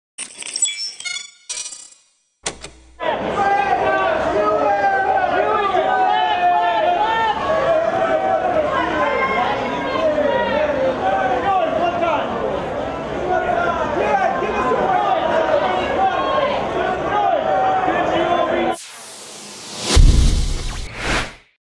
Ewan McGregor "Solo: A Star Wars Story" World Premiere Red Carpet
Broll footage: Ewan McGregor walks the red carpet at the world premiere of “Solo: A Star Wars Story” held at the TCL Chinese and Dolby Theaters in Los Angeles, California USA on May 10, 2018